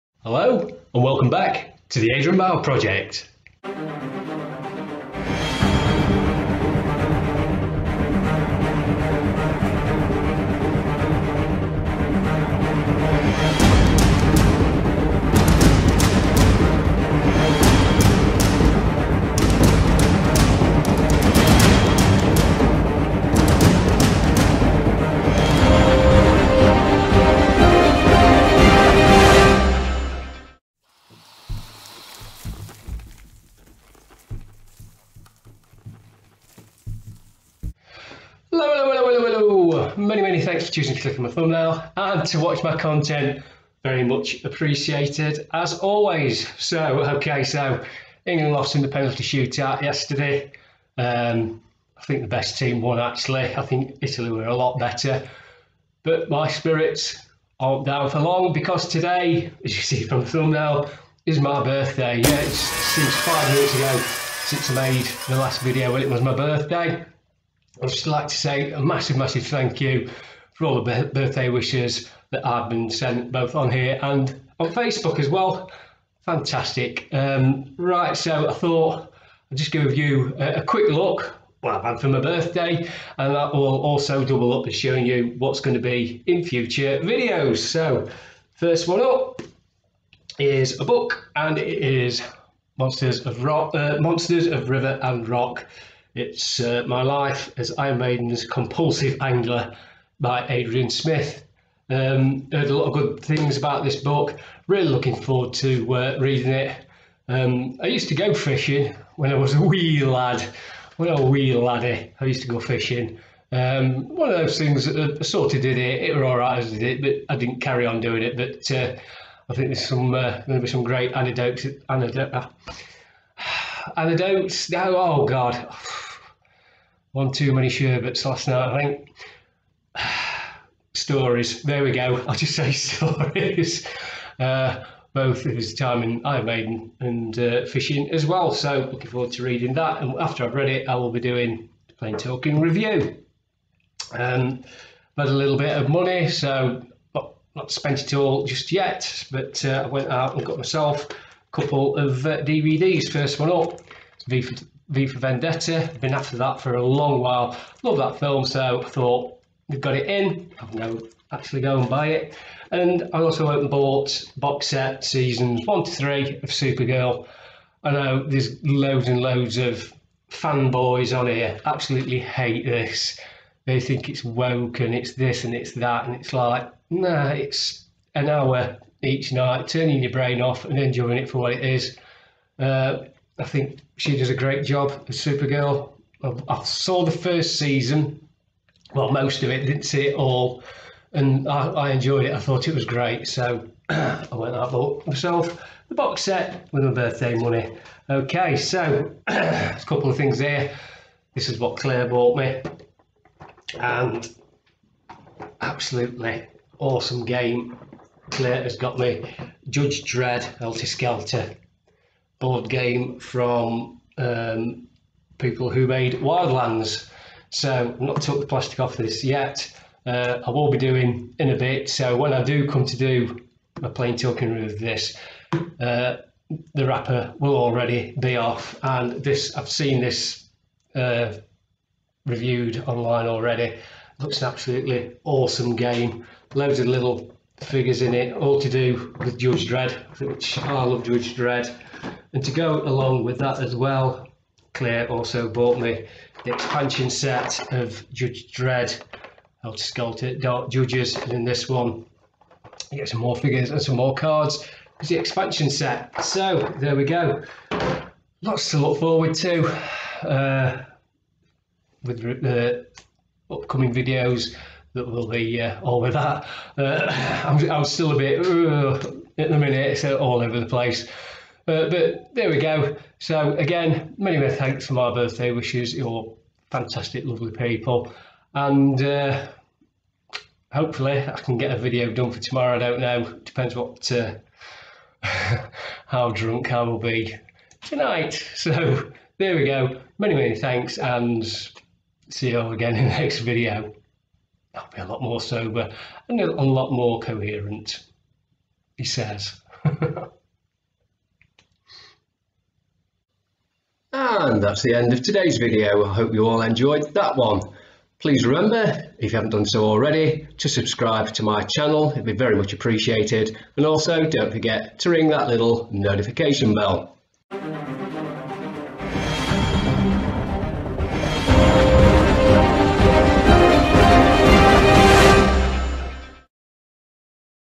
YEE-HAW!! I'm 57 Today!!! | What Pressies Did I Get?
On today's episode, I'm celebrating my 57th birthday, and letting you have a peek at what I got, which also culminates into a 'future videos' video.